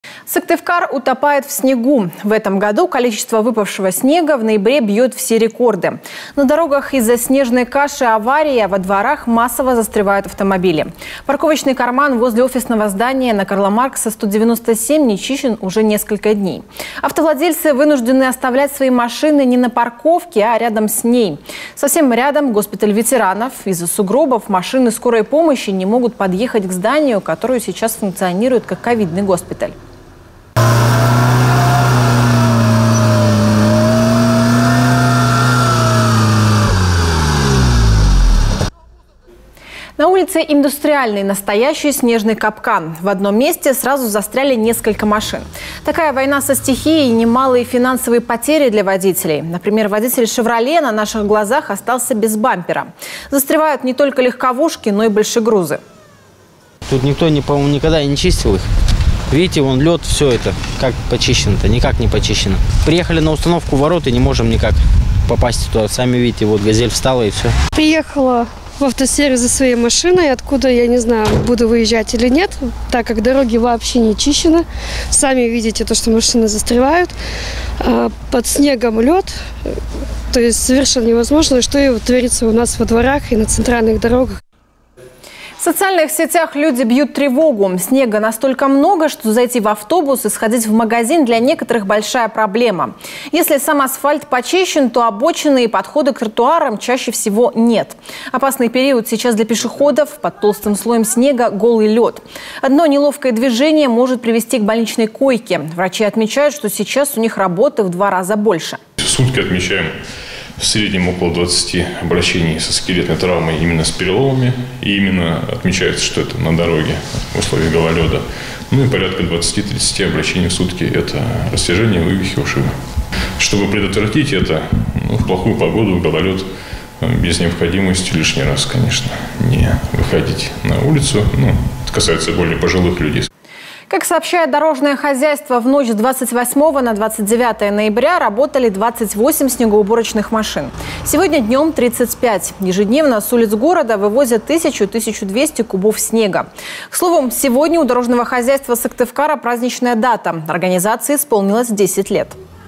Сыктывкар утопает в снегу.
В этом году количество выпавшего снега в ноябре бьет все рекорды. На дорогах из-за «снежной каши» - аварии, а во дворах массово застревают автомобили. «Парковочный карман» возле офисного здания на Карла Маркса, 197 не чищен уж несколько дней. Автовладельцы вынуждены оставлять свои машины не на парковке, а рядом с ней.